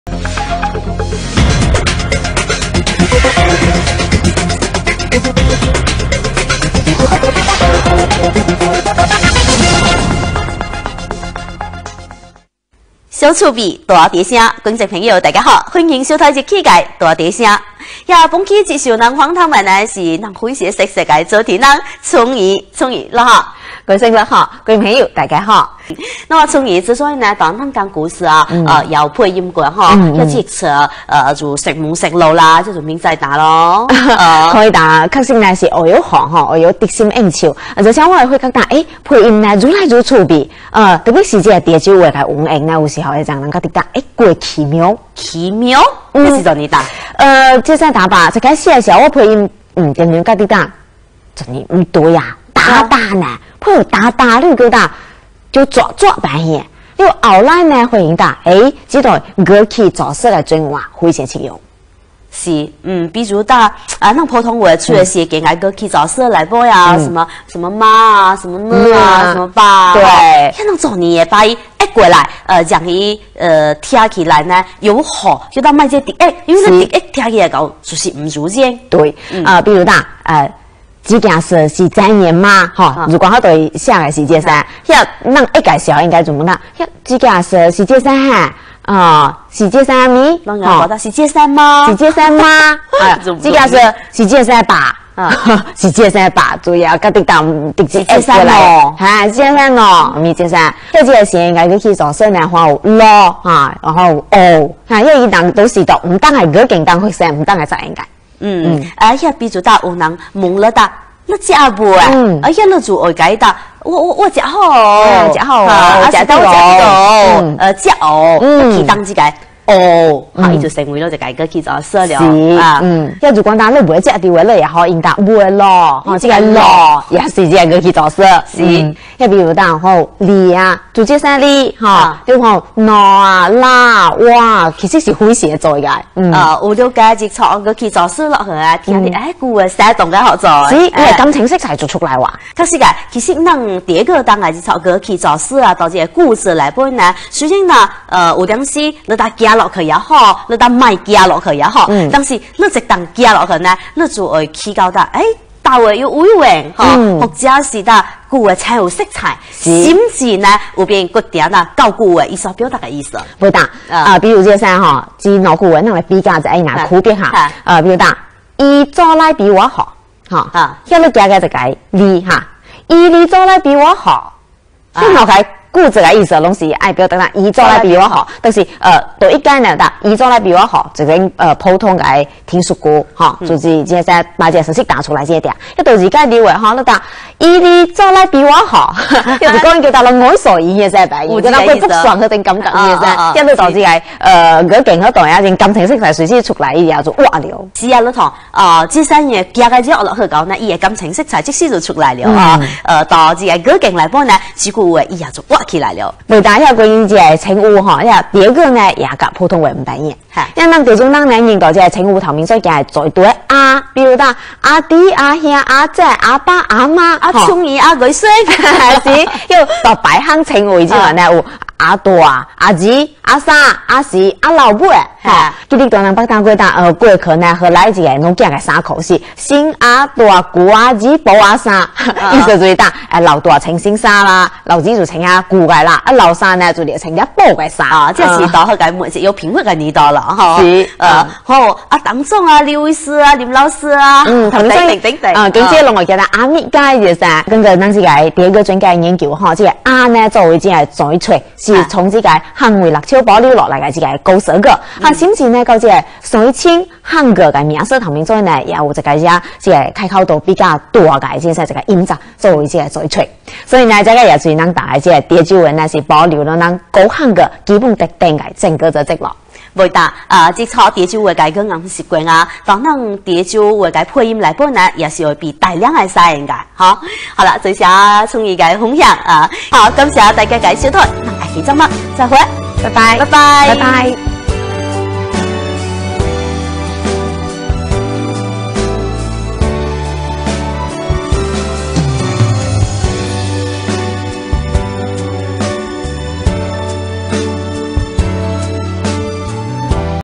大潮声 181024 春燕
潮州方言词
【灱灱】da da 本义干燥，这里指呆板不生动
【灱】da干

【绌绌】zuah zuah 缺少某些东西
【绌】zuah 缺、差、欠

〖语气助词〗ghw ki zo sw

【这件事是这样生吓】zi gian sw si zien sen hen 这件事是这样吗
〖这样〗zi ien 合音成zien 也读zie

【是这样生阿唔是】si zien sen a mi是不是这样
【阿】a 还是，如【来阿唔来】lai a m lai来还是不来
【唔是】m si合音成mi不是

【是这样生喏】si zien sen no 是这样吧

【我食好了】ua ziah ho ou 我吃了
【了】liou 这里掉落前部分li，变成ou
【食了】ziah ou 吃了

【未啰】bhue lo 还没有

【就这样生哩】zu zien sen li就是这样

【伊做来比我好】i zo lai bi ua ho 他做得比我好（普通陈述句）
【伊哩做来比我好】i li zo lai bi ua ho他做得比我好（陈述句 说话者表不满）

前缀“阿”的用法
“阿”作为前缀，在古汉语已经出现，潮州话还保留这种用法，不愧是古汉语的活化石。“阿”在潮州话有以下几种用法
1. 亲属称谓之前
【阿弟 阿兄 阿姐 阿爸 阿妈】a di a hian a ze a ba a ma 弟弟 哥哥 姐姐 爸爸 妈妈
2. 名字之前
【阿春燕 阿艺璇】a cung in a ghoi sueng
3. 家中排行
【阿大 阿二 阿三 阿四 阿老尾】a dua a ri a san a si a lao bhue 老大 老二 老三 老四 老幺
4. 职称之前
【阿陈总 阿李医师 阿林老师】a dang zong a li ui sw a lim lao sw

【新阿大 旧阿二 补阿三】sing a dua gu a ri bou a san老大穿新衣，老二穿旧衣，老三穿补过的衣，这是指过去物质贫乏时期的情况